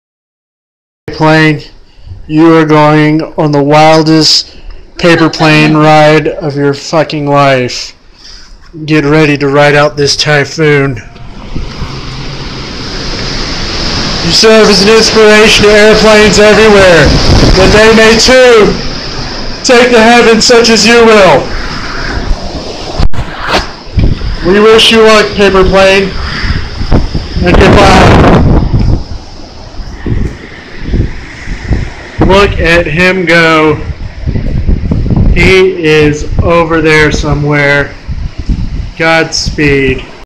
Paper plane rides super typhoon Neguri
Paper plane powered by Honda takes its place among the divine winds of Okinawa